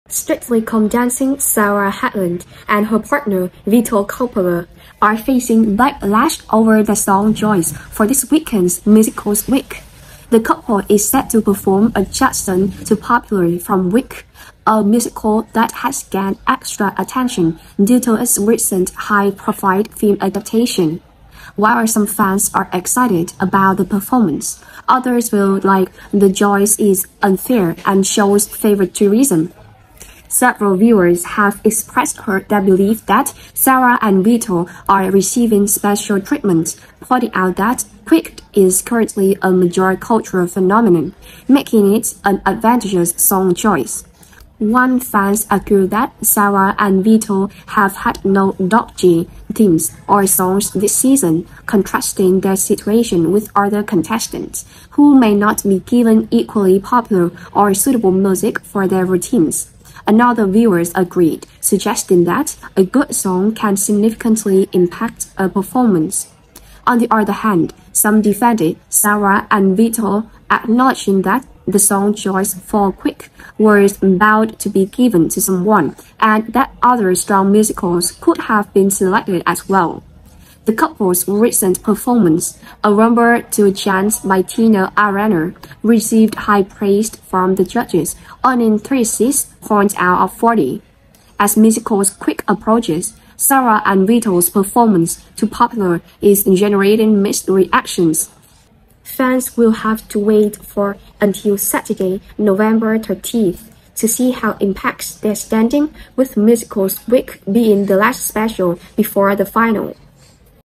Strictly Fans Cry Favouritism Over Sarah and Vito’s Song Choice
Fans accuse Strictly of favoritism over Sarah and Vito’s song choice for Musicals Week performance.